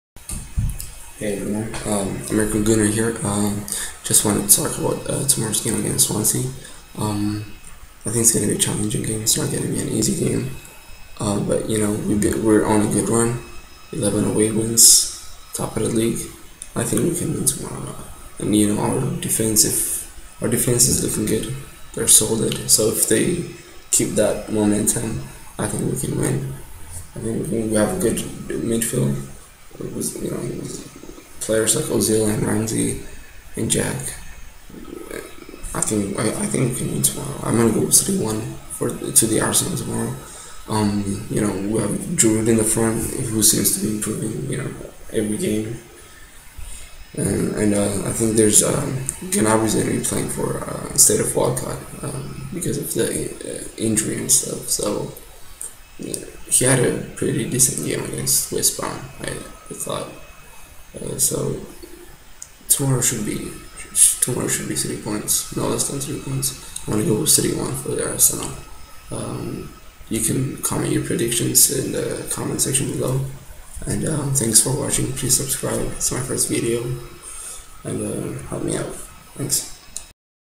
american gunner, arsenal vs swansea
a look at the arsenal squad before the Swansea game.
sorry about the video quality its my first you tube video.